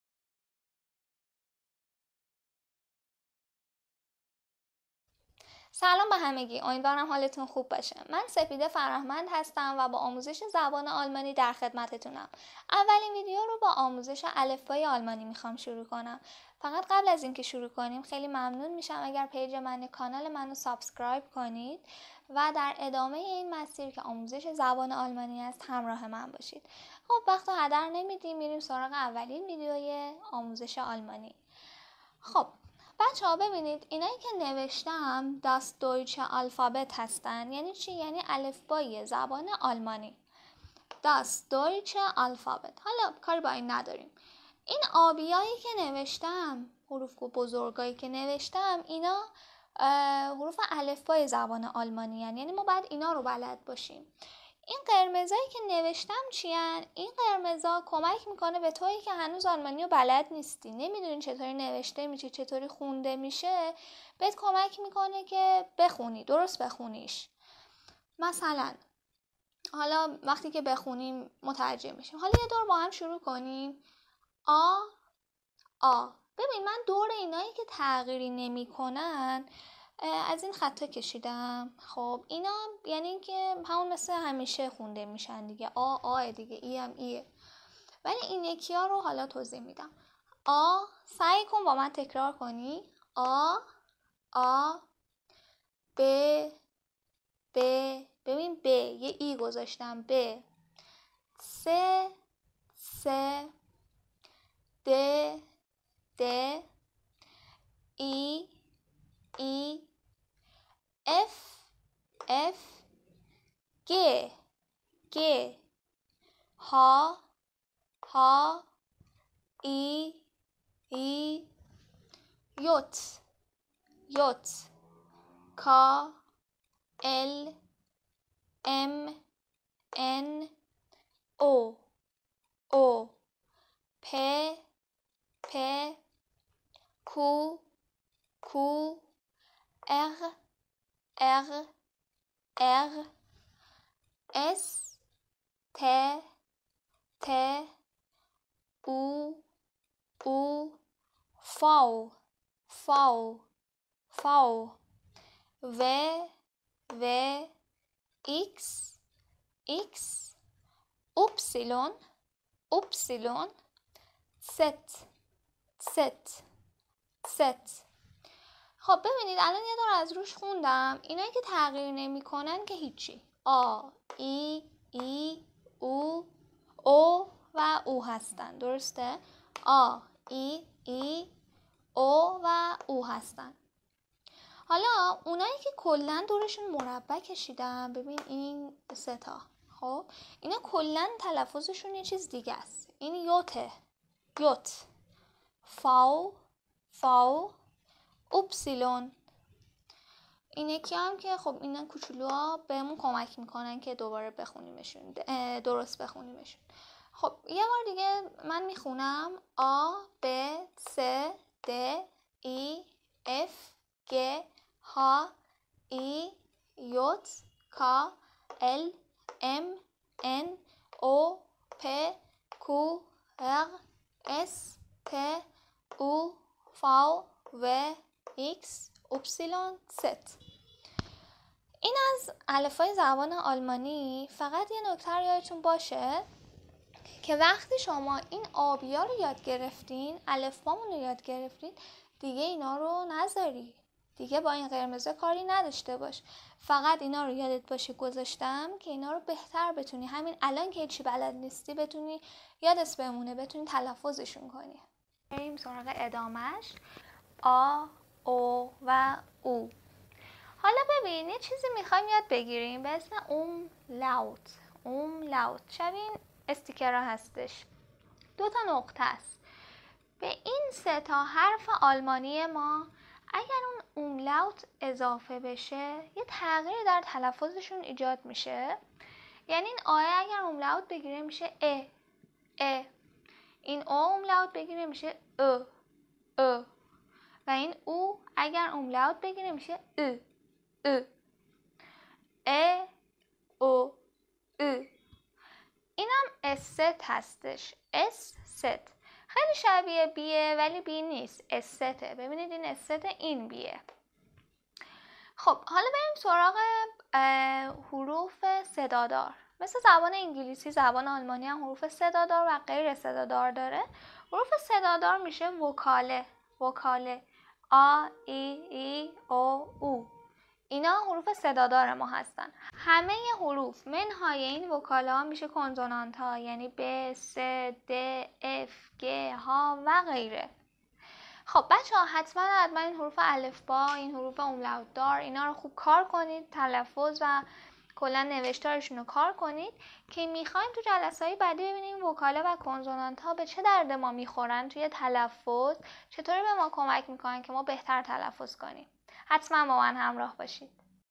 آموزش الفبای زبان آلمانی German language training
سلام دوستان
تو این ویدیو الفبای زبان آلمانی و یاد میگیریم
حتما کانال و سابسکرایب کنید چون کلی آموزش عالی در راهه...
Instagram.com/sepidehfarahmandd